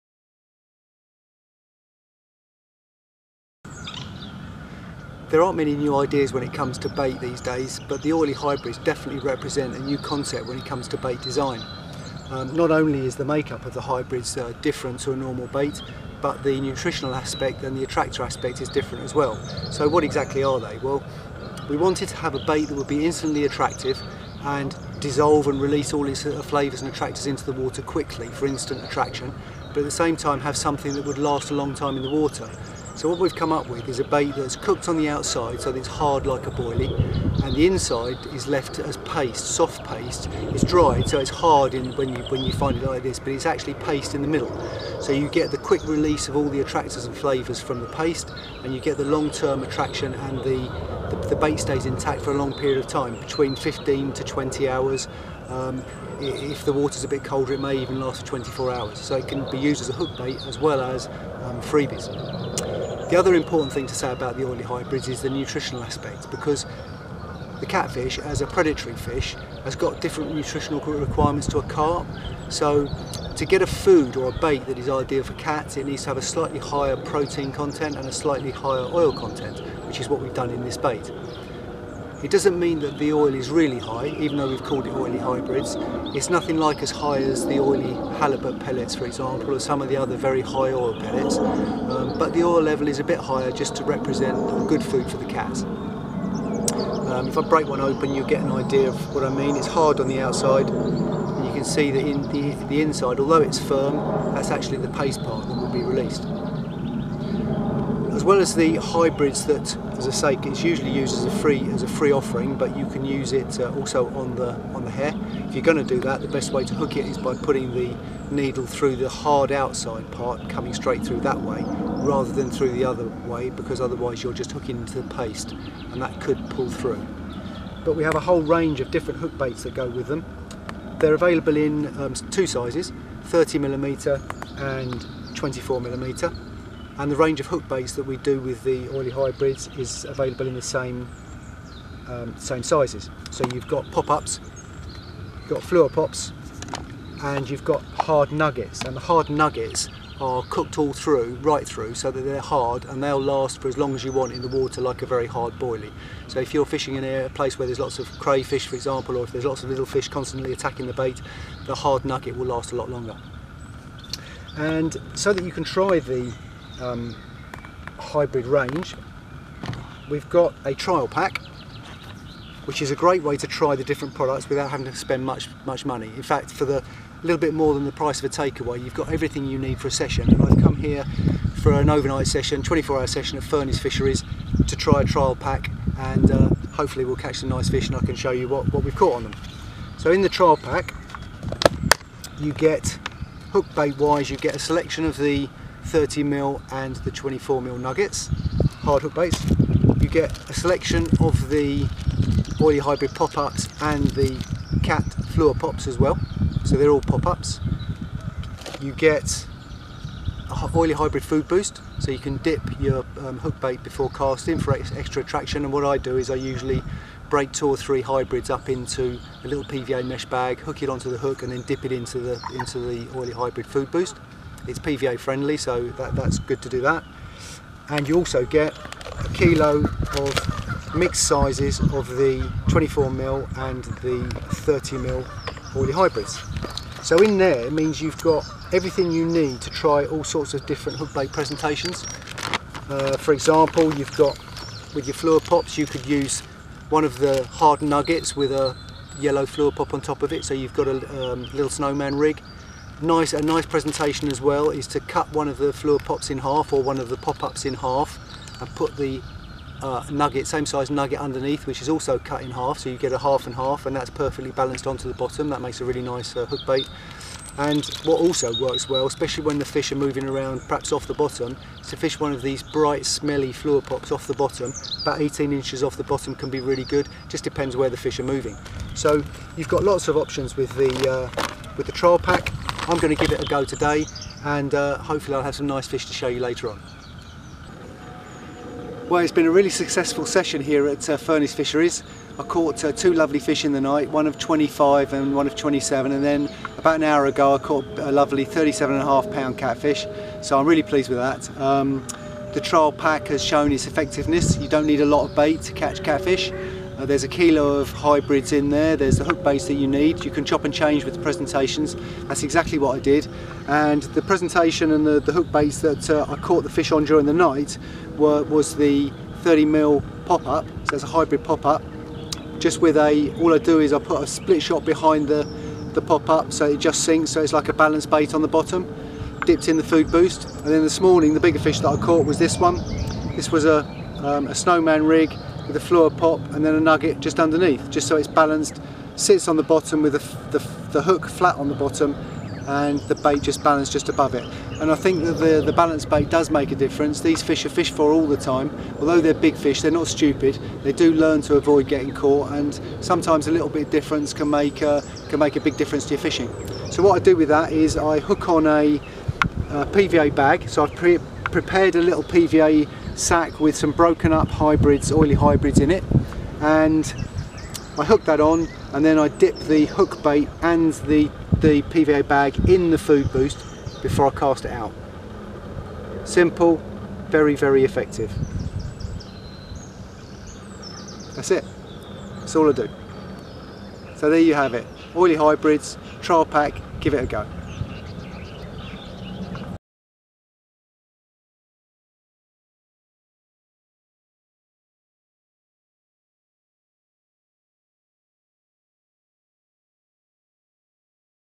Big Cat - Oily Hybrid Trial Pack
Trial Pack Big Cat Oily Hybrid baits. All you need to try the Oily Hybrid range in a convenient bucket.